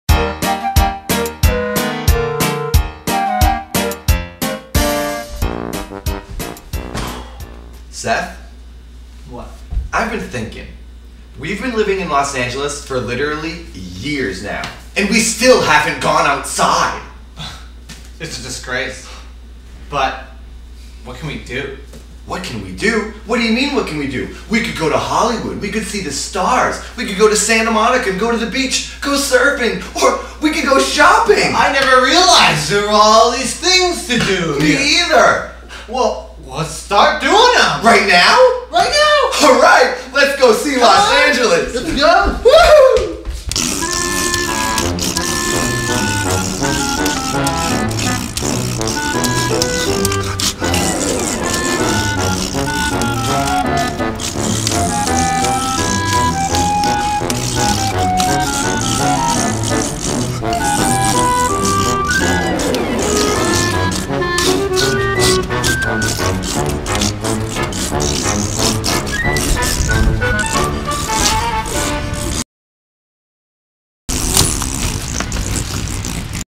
Nathan & Seth Enjoy Los Angeles
Nathan & Seth finally get out and enjoy life in Los Angeles.